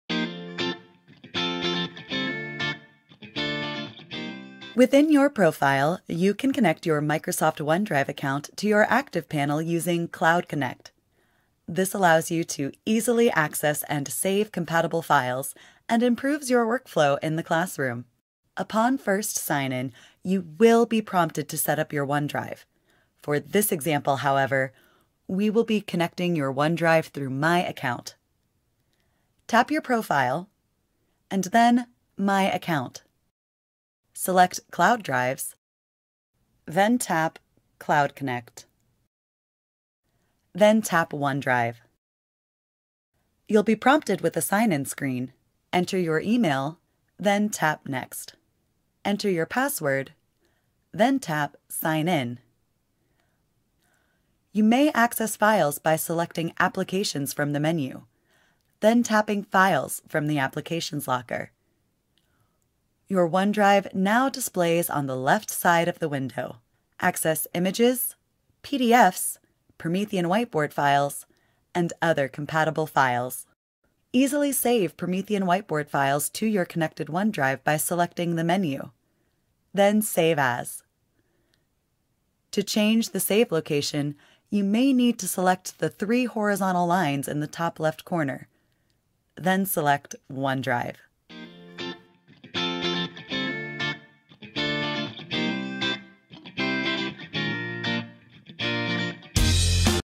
Cloud Connect Microsoft OneDrive
Learn how to use your existing resources on the Promethean ActivPanel by connecting your Microsoft OneDrive account.